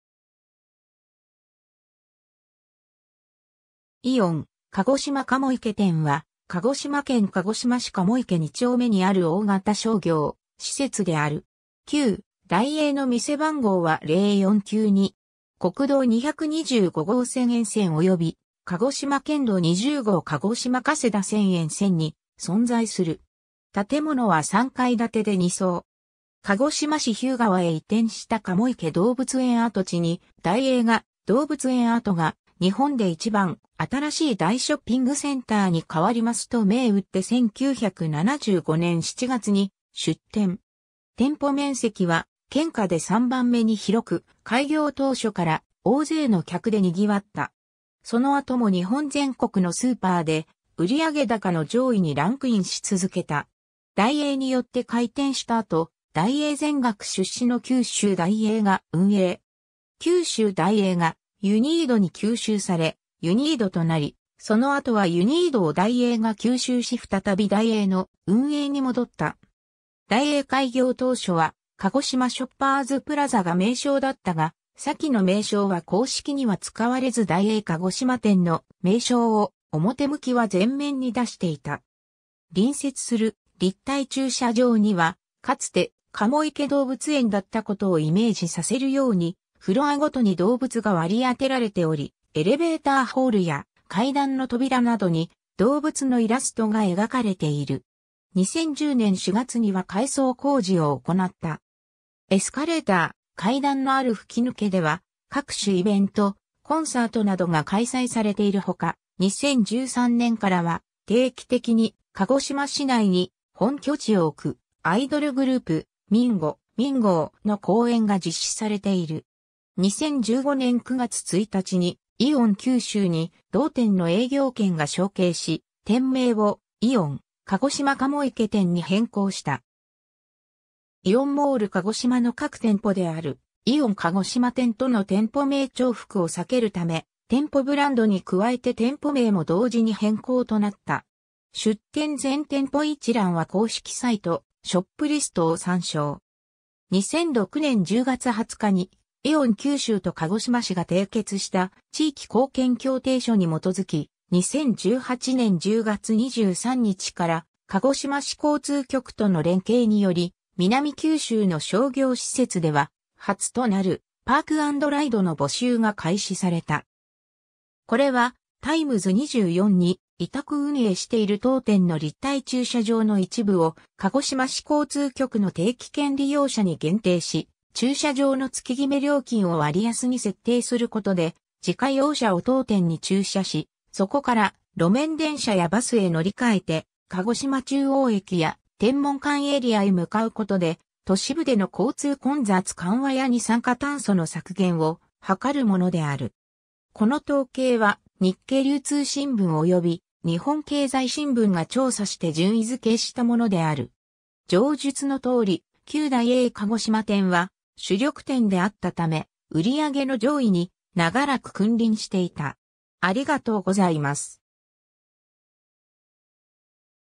イオン鹿児島鴨池店
イオン鹿児島鴨池店（イオンかごしまかもいけてん）は鹿児島県鹿児島市鴨池二丁目にある大型商業施設（ショッピングセンター）である。
旧ダイエーの店番号は0492。
 国道225号線沿線及び鹿児島県道20号鹿児島加世田線沿線に存在する。
建物は3階建てで二層。
鹿児島市平川へ移転した鴨池動物園跡地にダイエーが「動物園跡が日本で一番新しい大ショッピングセンターに変わります」と銘打って1975年（昭和50年）7月に出店。
店舗面積は県下で3番目に広く、開業当初から大勢の客で賑わった。
その後も日本全国のスーパーで売上高の上位にランクインし続けた（後述）。
 ダイエーによって開店した後、ダイエー全額出資の九州ダイエーが運営。
九州ダイエーがユニードに吸収されユニードとなり、その後はユニードをダイエーが吸収し再びダイエーの運営に戻った。
 ダイエー開業当初は鹿児島ショッパーズプラザが名称だったが、左記の名称は公式には使われずダイエー鹿児島店の名称を表向きは前面に出していた（ただし、鹿児島市への届出上のSC名称では鹿児島ショッパーズプラザとなっている）。
 隣接する立体駐車場には、かつて鴨池動物園だったことをイメージさせるようにフロアごとに動物が割り当てられており、エレベーターホールや階段の扉などに動物のイラストが描かれている。
 2010年4月には改装工事を行った。
 エスカレーター、階段のある吹き抜けでは各種イベント、コンサートなどが開催されているほか、2013年からは定期的に鹿児島市内（高見馬場）に本拠地を置くアイドルグループ「MINGO!×MINGO!」の公演が実施されている。
 2015年9月1日にイオン九州に同店の営業権が承継し、店名を「イオン鹿児島鴨池店」に変更した。
イオンモール鹿児島の核店舗である「イオン鹿児島店（旧・ジャスコ鹿児島店）」との店舗名重複を避けるため、店舗ブランドに加えて店舗名も同時に変更となった。
 2006年10月20日にイオン九州と鹿児島市が締結した地域貢献協定書に基づき、2018年10月23日から鹿児島市交通局との連携により、南九州の商業施設では初となるパークアンドライドの募集が開始された。
 これは、タイムズ24に委託運営している当店の立体駐車場の一部を鹿児島市交通局の定期券利用者に限定し、駐車場の月極料金を割安（全日定期とより割安な平日定期がある）に設定することで、自家用車を当店に駐車し、そこから路面電車やバスへ乗り換えて鹿児島中央駅や天文館エリアへ向かうことで、都市部での交通混雑緩和や二酸化炭素の削減を図るものである。
 この統計は、日経流通新聞及び日本経済新聞が調査して順位付けしたものである。
上述の通り、旧ダイエー鹿児島店は、主力店であった為、売上の上位に長らく君臨していた。